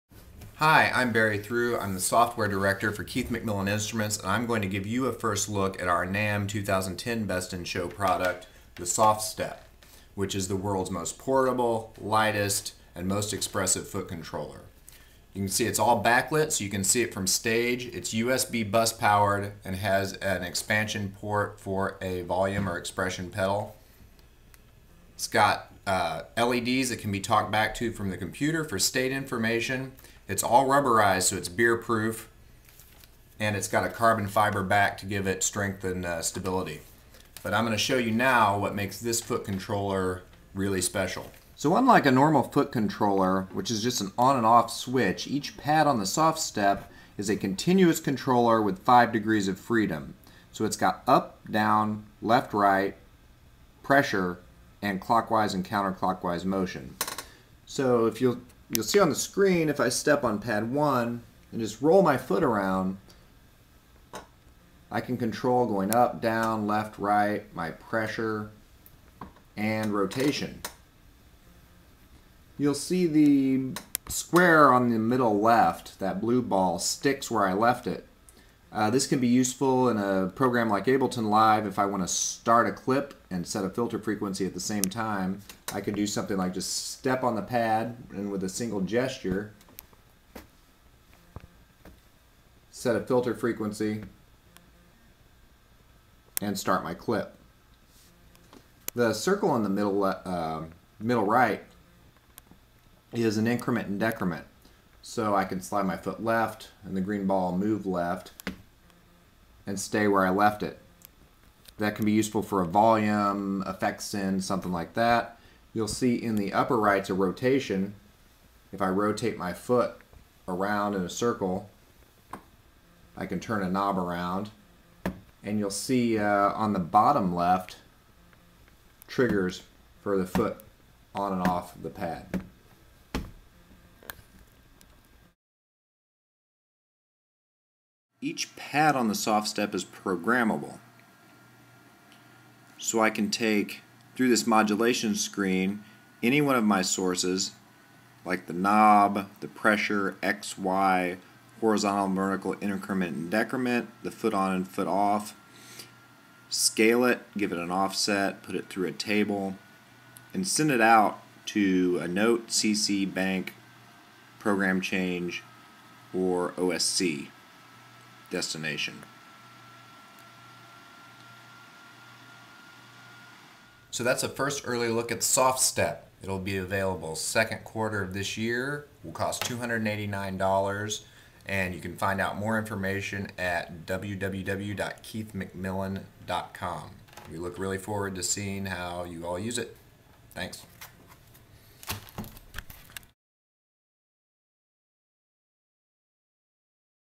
SoftStep Preview
This is a first look at the new SoftStep continuous foot controller from Keith McMillen Instruments. The SoftStep is a lightweight fully programmable USB foot controller with 5 degrees of freedom on each key.